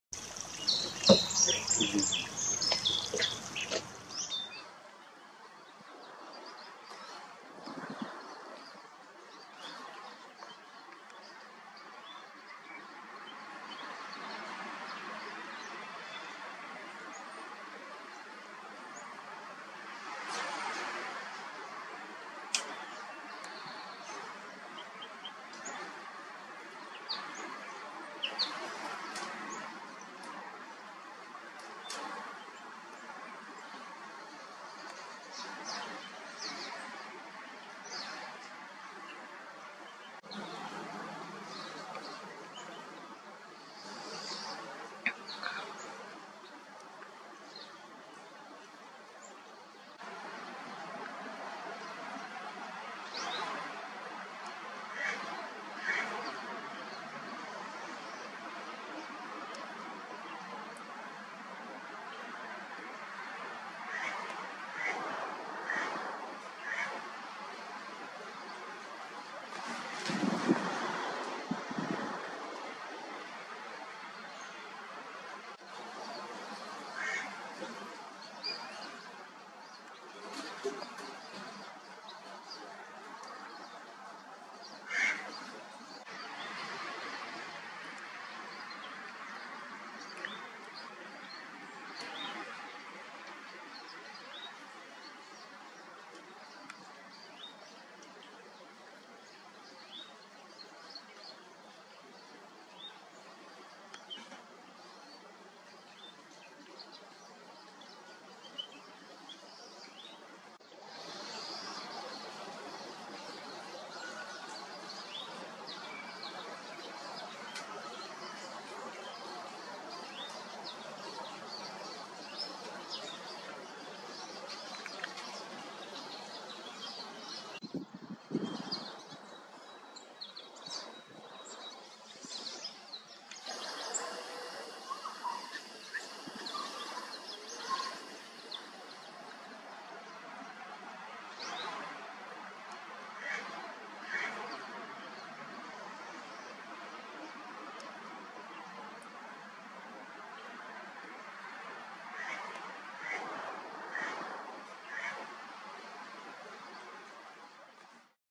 Piranga écarlate / Scarlet Tanager
Piranga écarlate anciennement appelé Taranga écarlate. Chez nous.

Scarlet Tanager in our backyard.